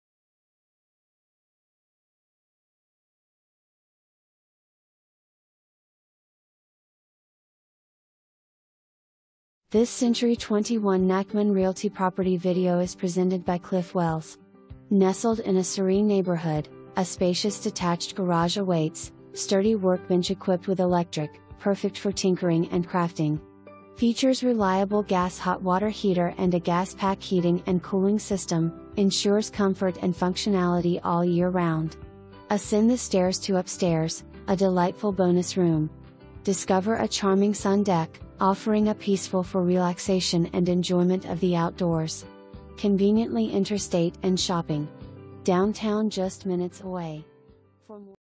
CENTURY 21 Nachman Realty - 2420 Spruce St
Please enjoy this property video created by CENTURY 21 Nachman Realty

NESTLED IN A SERENE NEIGHBORHOOD, A SPACIOUS DETACHED GARAGE AWAITS, STURDY WORKBENCH EQUIPPED WITH ELECTRIC, PERFECT FOR TINKERING AND CRAFTING. FEATURES RELIABLE GAS HOT WATER HEATER AND A GAS PACK HEATING AND COOLING SYSTEM, ENSURES COMFORT AND FUNCTIONALITY ALL YEAR ROUND. ASCEND THE STAIRS TO UPSTAIRS, A DELIGHTFUL BONUS ROOM. DISCOVER A CHARMING SUN DECK, OFFERING A PEACEFUL FOR RELAXATION AND ENJOYMENT OF THE OUTDOORS. CONVENIENTLY INTERSTATE AND SHOPPING. DOWNTOWN JUST MINUTES AWAY.

2 full bath

Cliff Wells
CENTURY 21 Nachman Realty